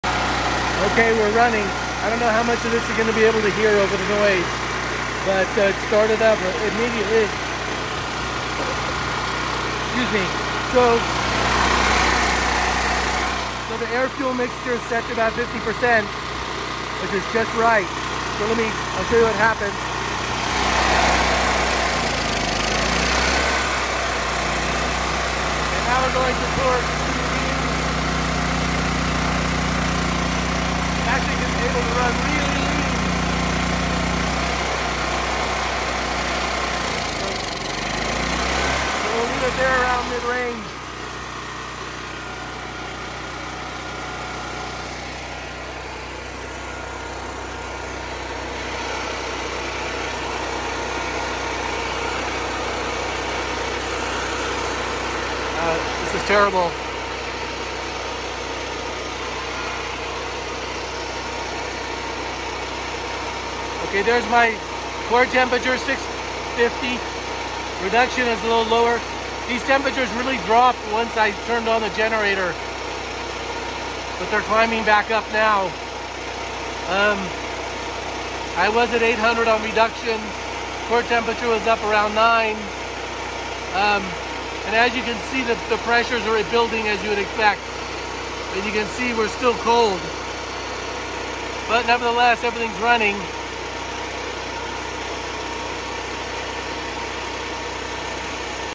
Gasifier Startup
Up and running. After period of warm up to allow reduction zone to reach around 500c (ideally) 800c, unit start right up